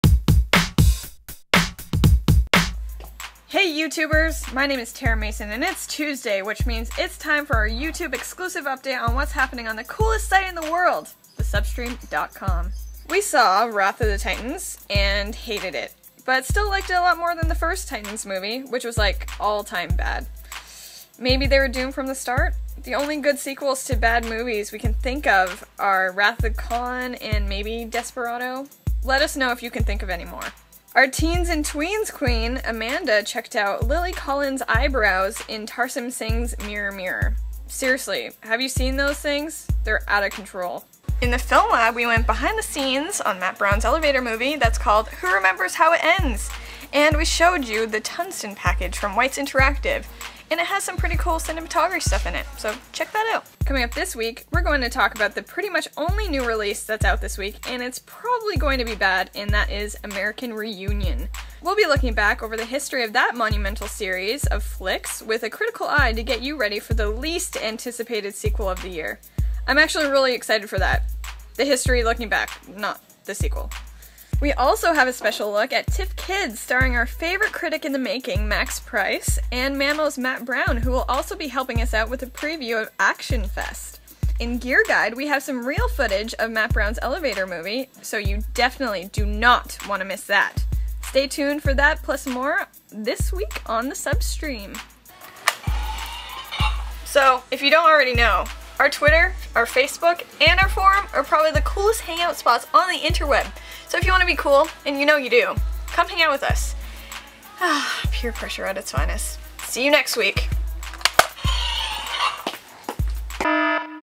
YOUTUBE EXCLUSIVE: thesubstream BEAT April 3, 2012
Coming up this week: The least anticipated sequel probably ever, American Reunion, a special look at TIFF Kids starring Max Price and more behind the scenes coverage of Matt Brown's elevator movie.